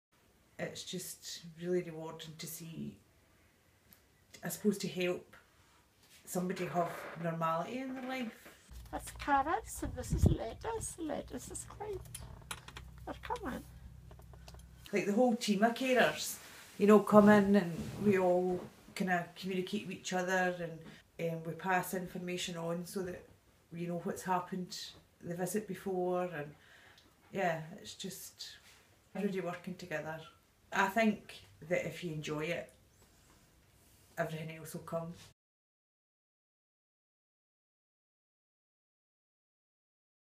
A career in home care - Bella Wishart
A short film featuring one of our home carers, Bella Wishart, talking about the rewards of the job.